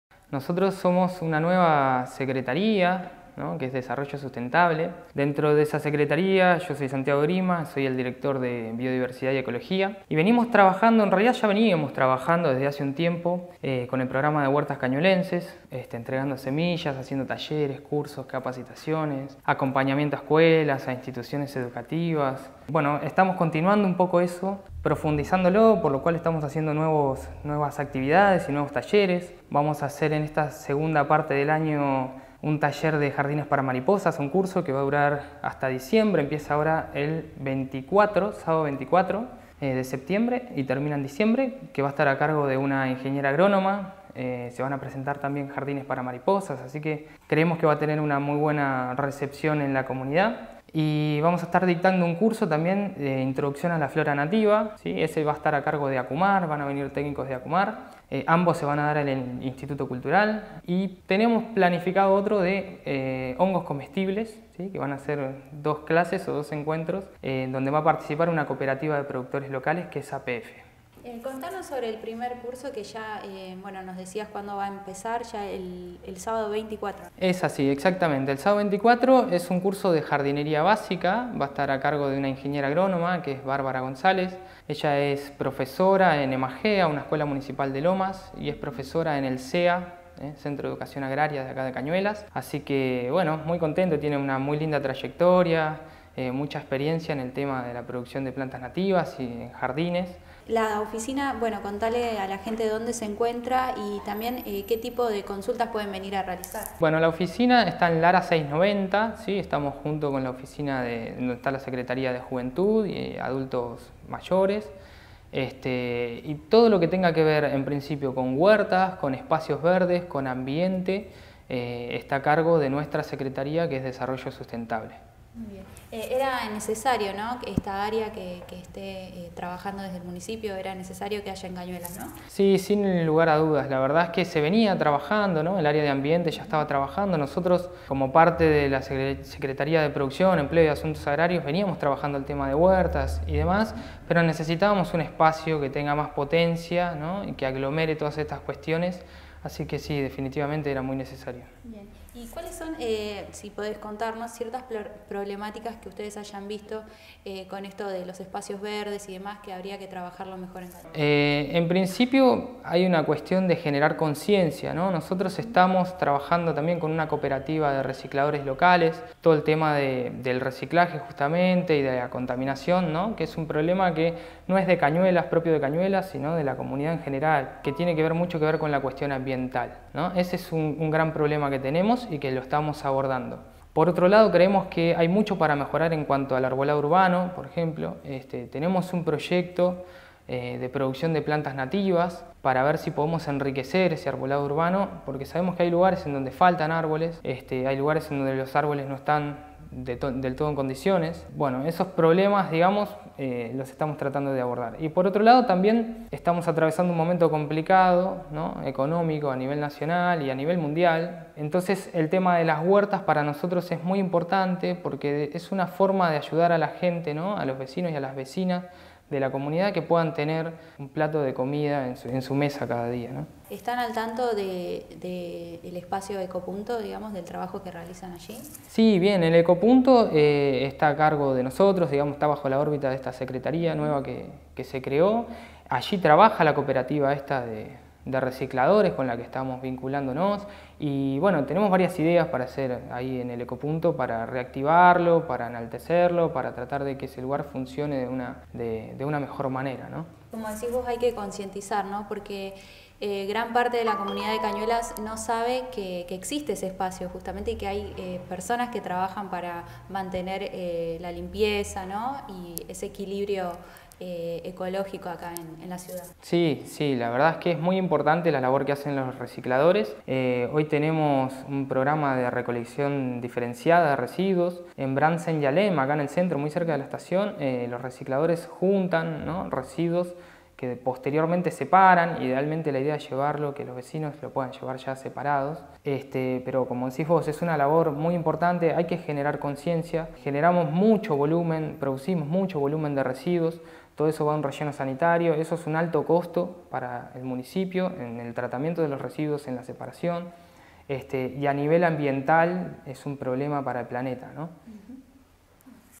Se puso en marcha una dirección municipal de Biodiversidad y Ecología
Tiene como fin brindar información sobre el sembrado de huertas propias, la separación de residudos y capacitar sobre la flora nativa y la jardinería conscientes.